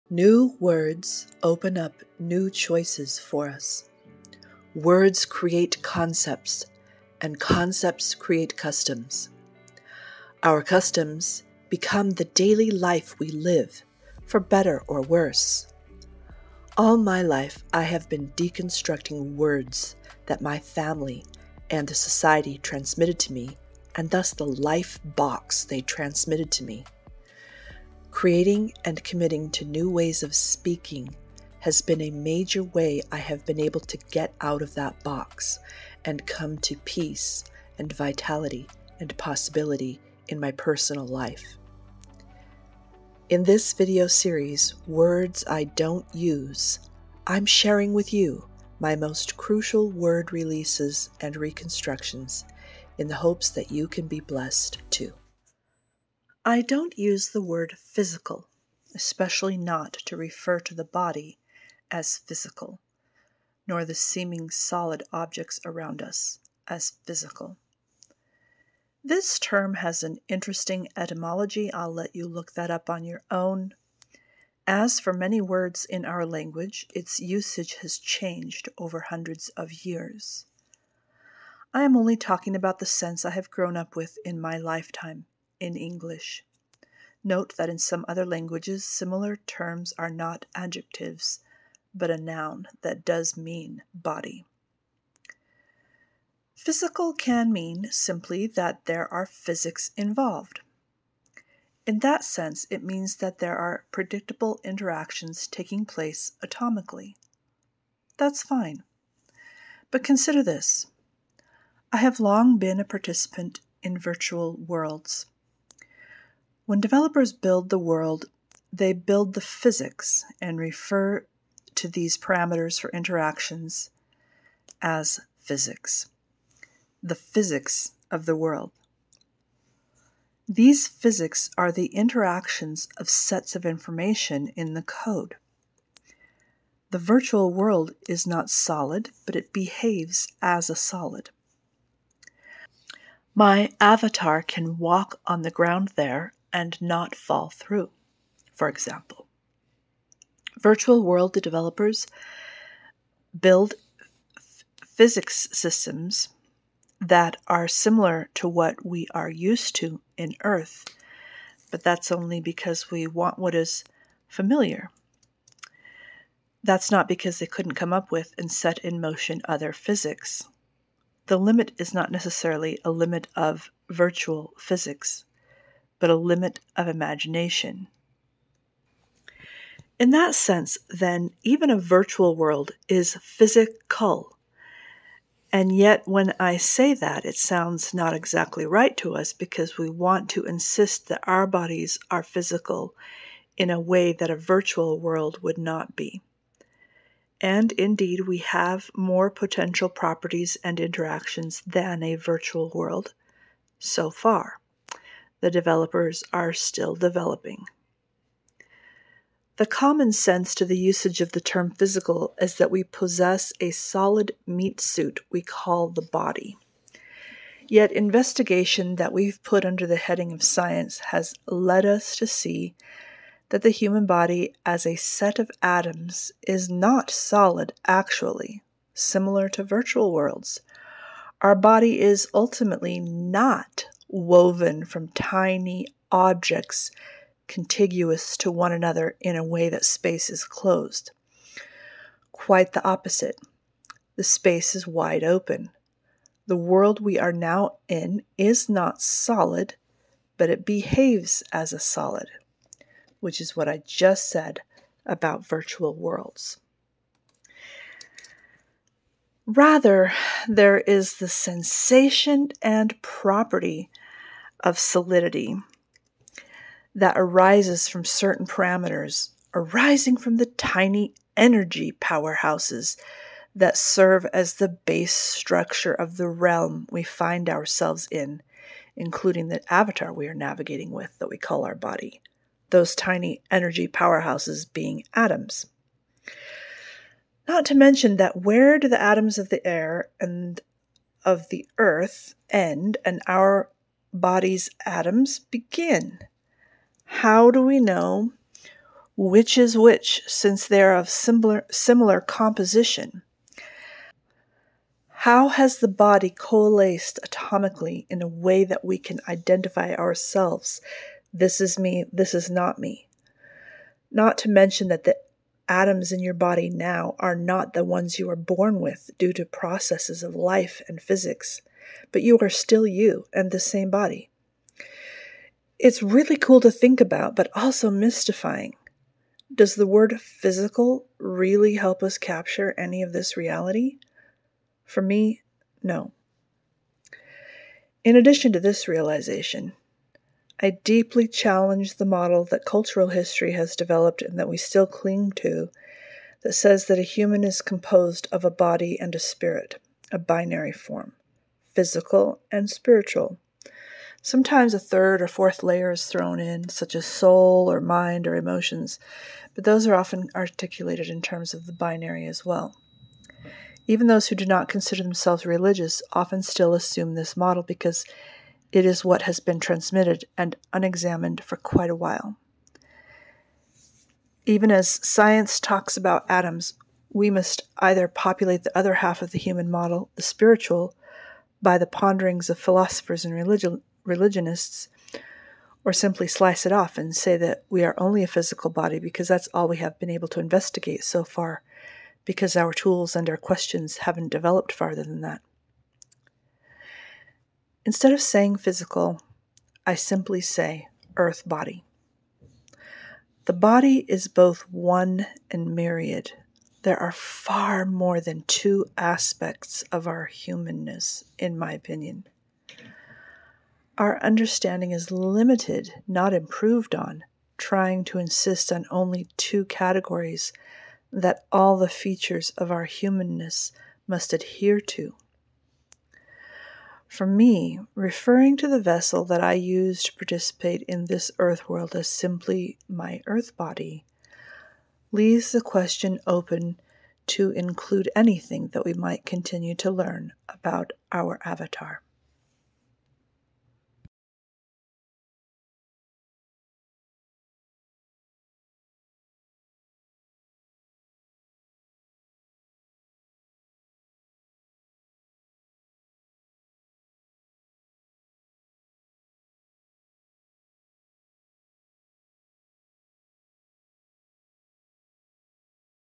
Physical (Words I Don't Use)
I don’t use the word “physical” especially not to refer to the body as “physical” nor the seeming solid objects around us as “physical”.

‘Physical’ can mean simply that there are physics involved. In that sense, it means that there are predictable interactions taking place atomically.

But consider this. I have long been a participant in virtual worlds. When developers build the world, they build the physics and refer to these parameters for interactions as ‘physics’; the ‘physics’ of the world. These physics are the interactions of sets of information in the code. The virtual world is not solid but it behaves as a solid.

The common sense to the usage of the term ‘physical’ is that we possess a solid meat suit we call the body. Yet, investigation that we’ve put under the heading of ‘science’ has led to us see that the human body as a set of atoms is not solid, actually.  The world we are now in is not solid, but it behaves as a solid.

There is the sensation and property of solidity that arises from certain parameters arising from the tiny energy powerhouses that serve as the base structure of the realm we find ourselves in, including the avatar we are navigating with (that we call our body). Those tiny energy powerhouses being atoms.

In addition to this realization, I deeply challenge the model that cultural history has developed and that we still cling to that says that a human is composed of a body and a spirit—a binary form. Physical and spiritual.

Instead of saying physical, I simply say Earth body. The body is both one and myriad. There are far more than two aspects of our humanness, in my opinion. Our understanding is limited, not improved on, trying to insist on only two categories that all the features of our humanness must adhere to. For me, referring to the vessel that I use to participate in this earth world as simply my Earth body leaves the question open to include anything that we might continue to learn about our avatar.